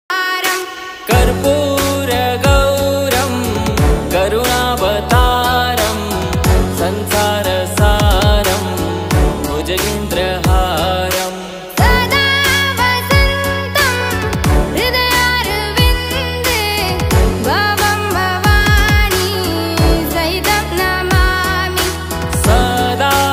python(ajagar) non venomous snake resque⚠️warning -- don't try this at own⚠️resque by trained person
Indian rock python snake (ajagar) non venomous snake resque at mind space project Nerul
⚠️ warning -- don't try this at own ⚠️
            resque by trained person